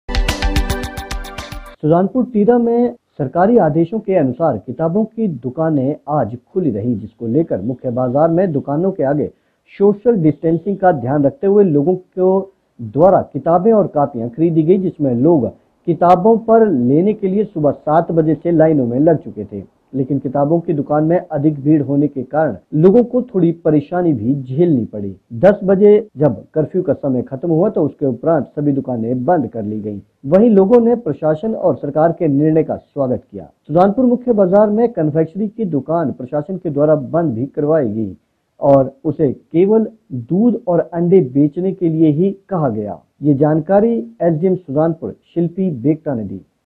11 april 17 सुजानपुर तिरा में उक्त आदेश के अनुसार किताबों की दुकान खुल चुकी
सुजानपुर तिरा में उक्त आदेश के अनुसार किताबों की दुकान खुल चुकी हैं जिसको लेकर मुख्य बाजार में दुकानों के आगे सोशल डिस्टेंसिंग का ध्यान रखते हुए लोगों के द्वारा किताब और कॉपियों की खरीद की गई जिसमें लोग किताब पर लेने के लिए सुबह 7:00 बजे से ही लाइन में लग चुके थे लेकिन किताबों की दुकानों में अधिक भीड़ होने के कारण लोग 10:00 बजे तक अपनी बारी का इंतजार करते रहे लेकिन 10:00 बजे दुकान में बंद होने के उपरांत उन्हें किताबें नहीं मिल पाई जिन्हें द्वारा कल सुबह लाइन में लगना पड़ेगा उन्होंने प्रशासन और सरकार के निर्णय का भी स्वागत किया है मुख्य बाजार में कन्फेक्शनरी की दुकान प्रशासन द्वारा बंद करवा दिया गया है जो लोग दूध और अंडा ही बेचते थे उन्हें अपनी दुकान का शटर बंद करने के उपरांत ही अंडा और दूध सकेंगे यह जानकारी अधिकारी शिल्पा द्वारा दी गई